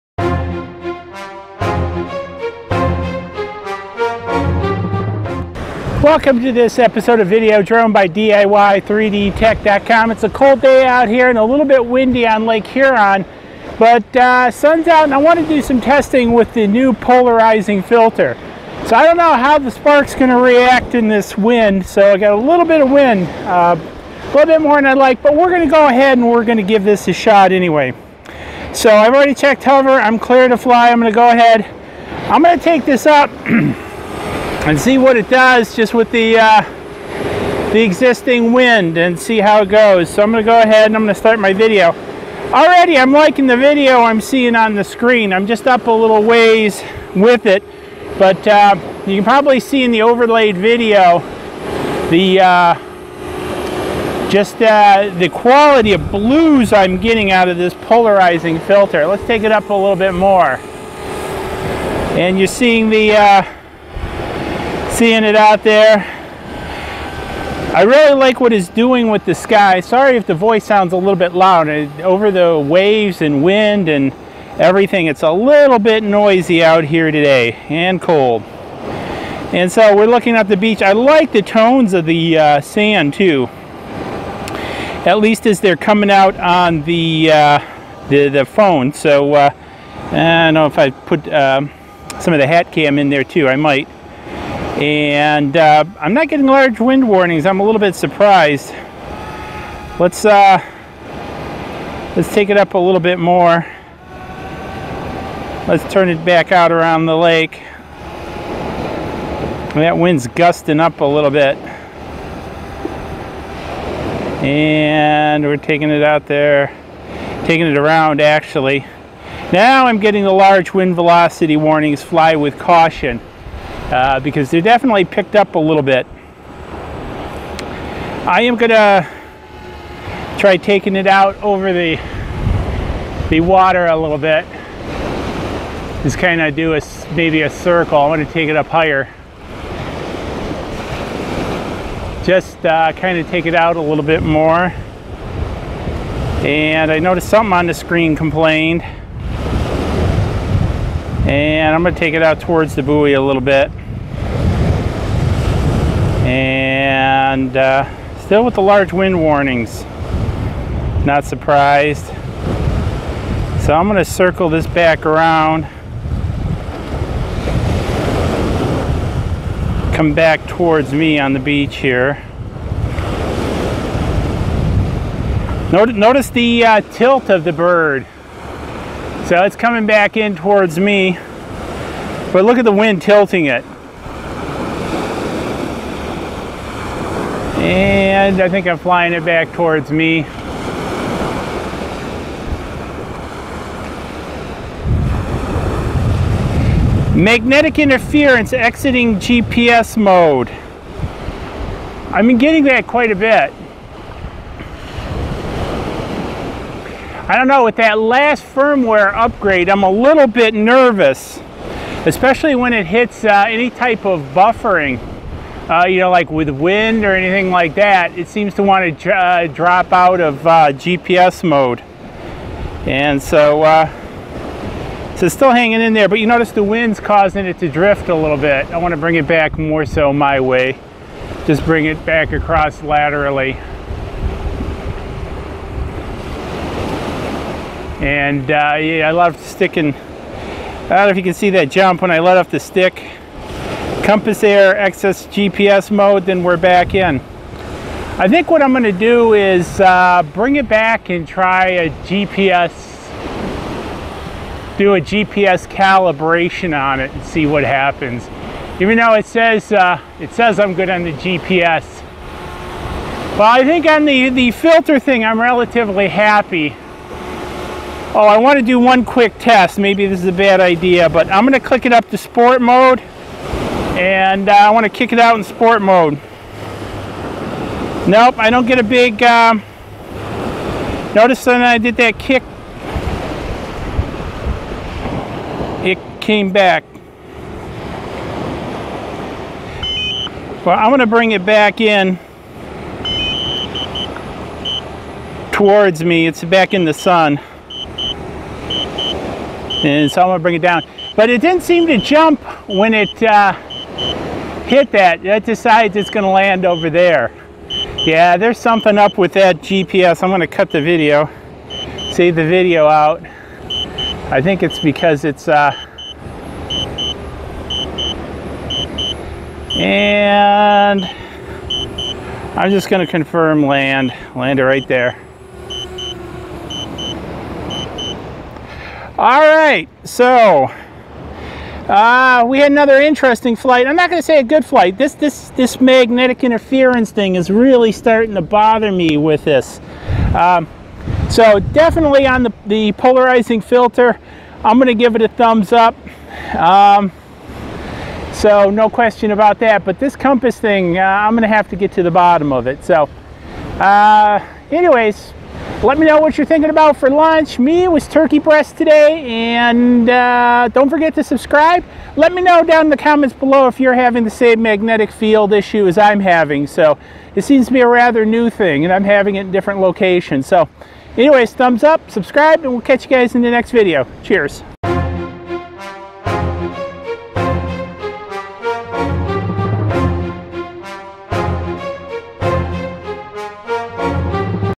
DJI Spark - Flying with an Off Brand CPL Filter Good or Bad!
We got in a new circular polarizing (CPL) filter for the DJI Spark from China so we are going to hit the breach an go test it out and see if it was worth it!  for more information on this and many other projects!

You can find this filter here along with many other for the DJI Spark

DJI Spark Links
DJI Spark Fly More Combo Packages:

Basic DJI Spark version without Remote: White